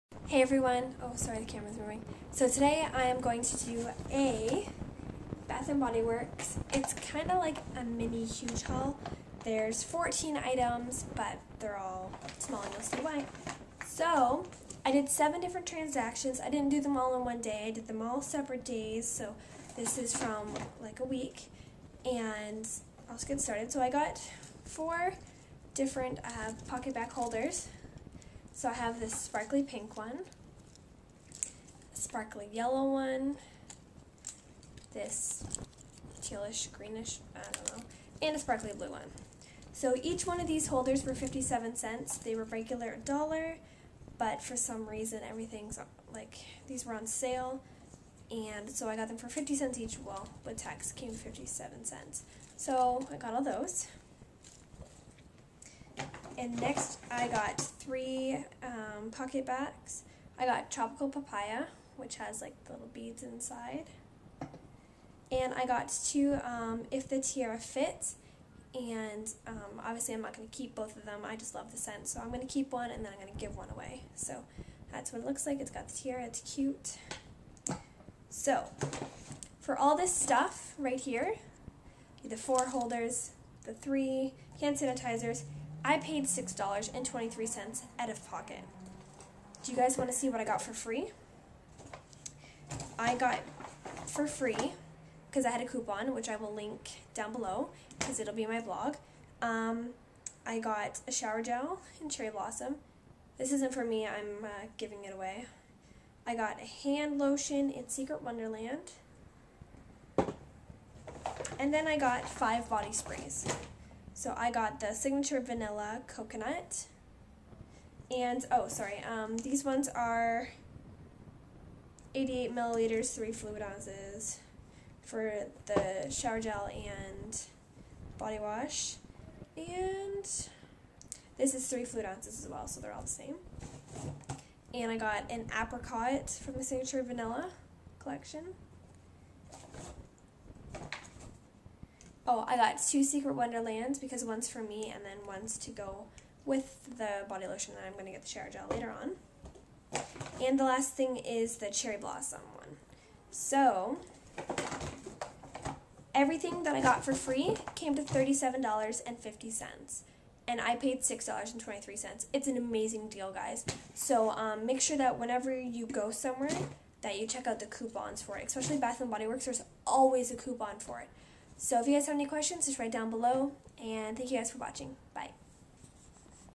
HUGE little Bath and Body Works Haul
Just some things that I got from Bath and Body Works all travel sized items were free.

Paid: $6.23
Got for free: $37.50